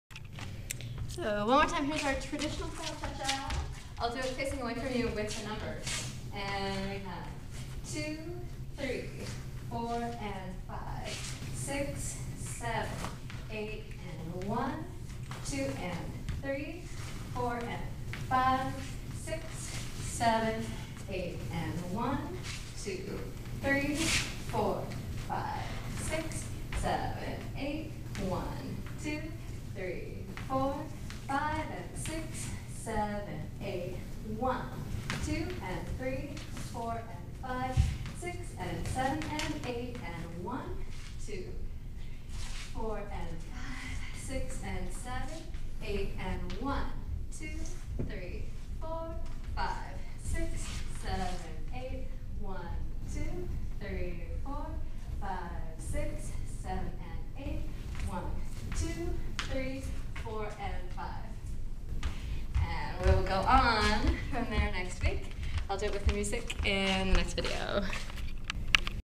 Latin Jazz trad cha cha counts
Dance steps for Latin Jazz class.  Check for a full schedule of classes in the San Francisco Bay Area.